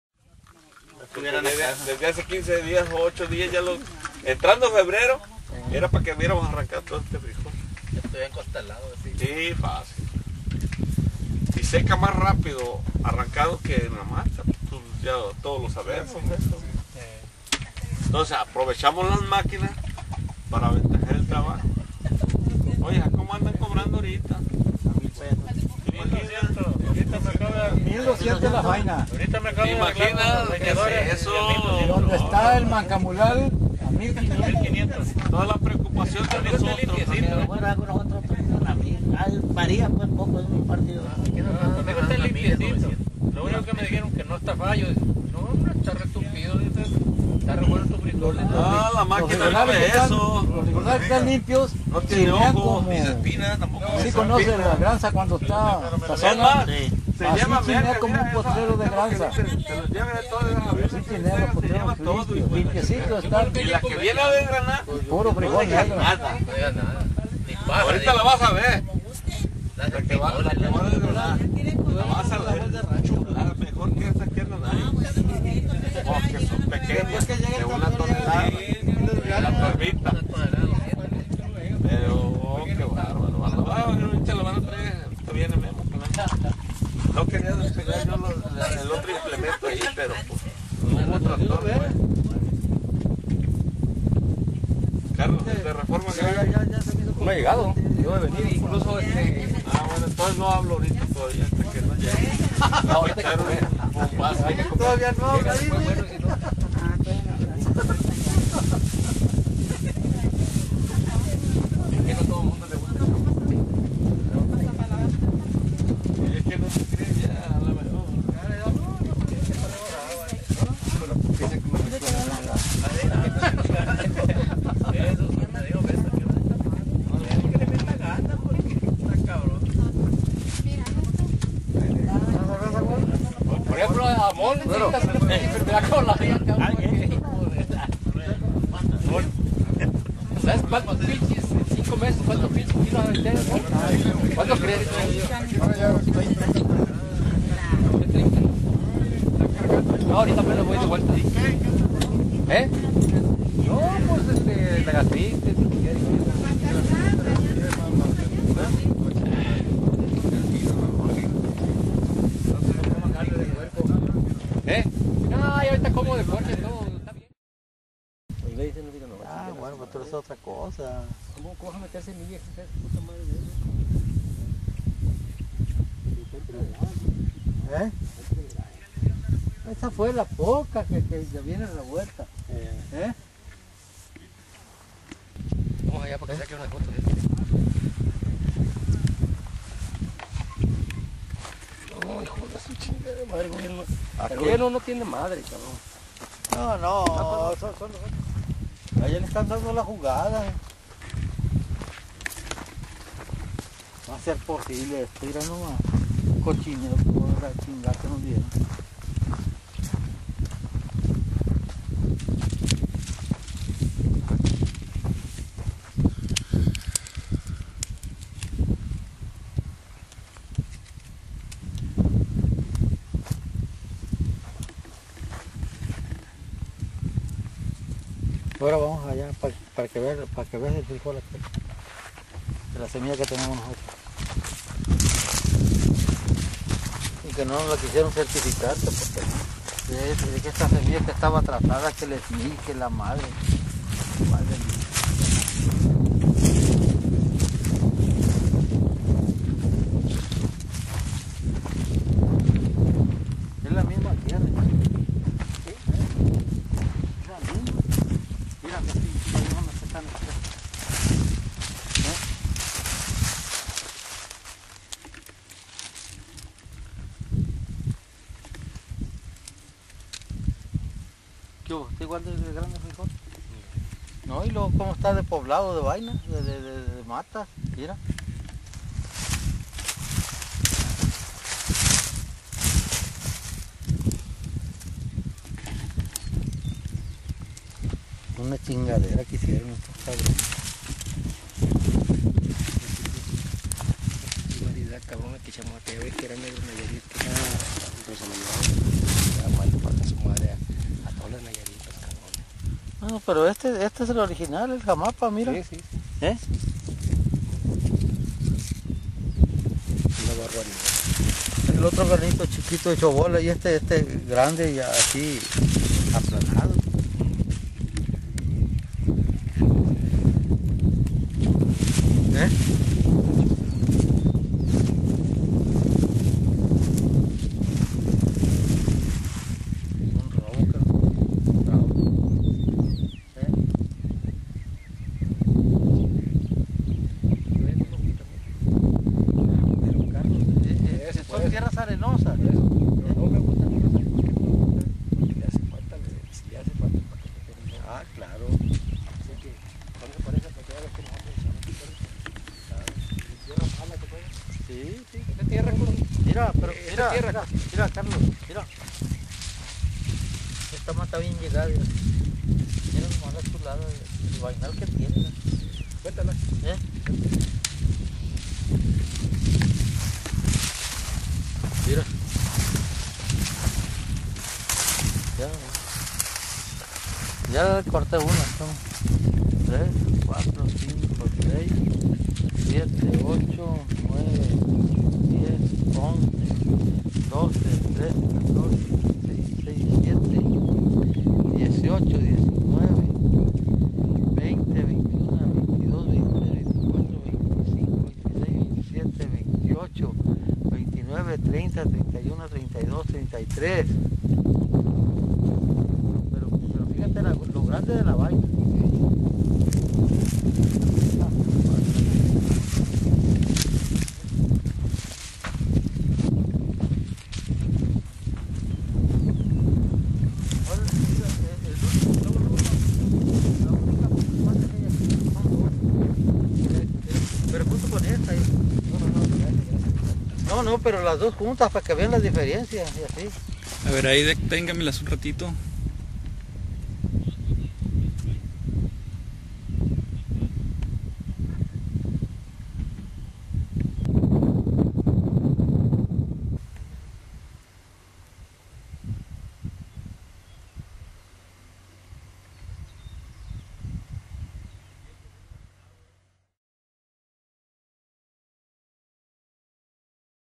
Aután
Los productores de frijol de Aután, Nayarit, denuncian el perjuicio de su cosecha al usar semilla proporcionada por el gobierno